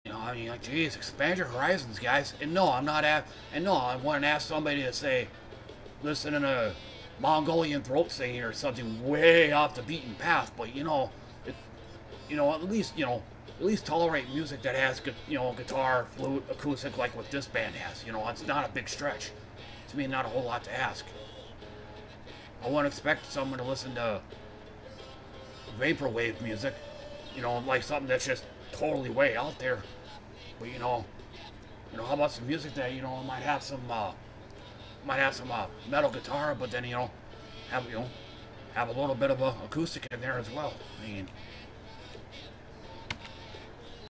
A quick rant about close-minded people.
A convo highlight about music "purists" who can't tolerate musical diversity, like Heart, Jethro Tull, ELO, etc.